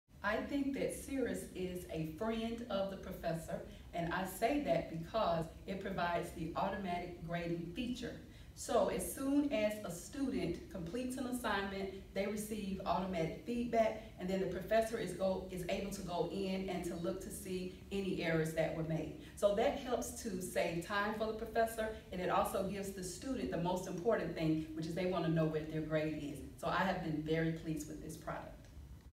How Cirrus Is A Friend to Professors
Mia Moore, an instructor at Clark Atlanta University in the department of Cyber-Physical Systems, explains how the Cirrus learning environment is a friend to professors.